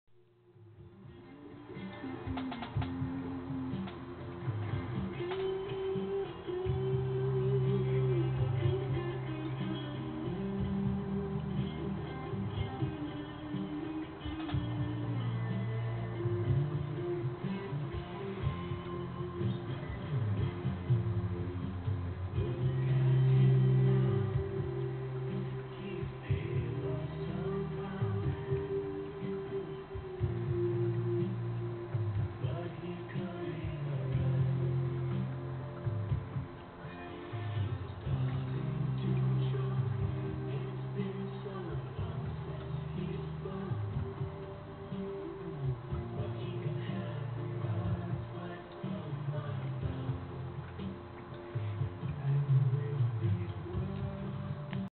Personal Pink Floyd Laser show
Playing with two new birthday toys, camera and the laser projector.